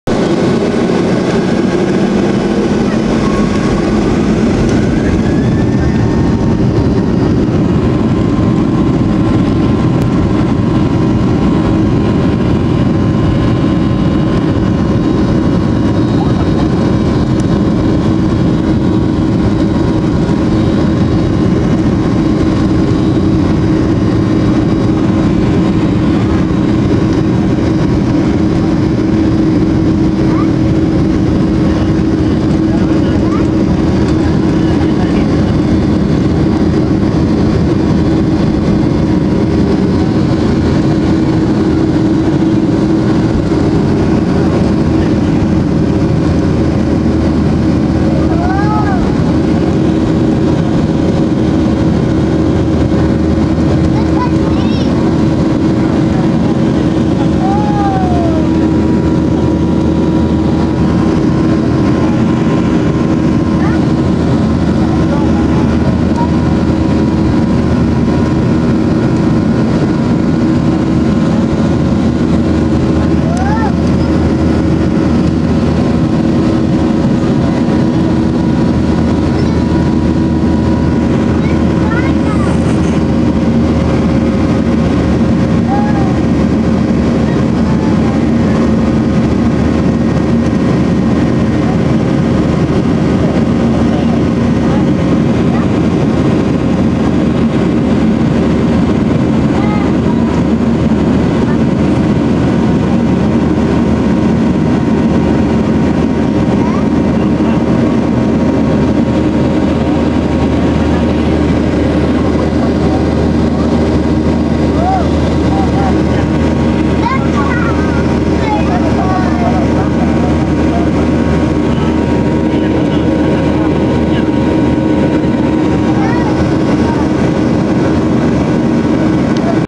Taking off from Miami. AA B737-800.
On my way back to El Salvador. Departing on AA flight 925.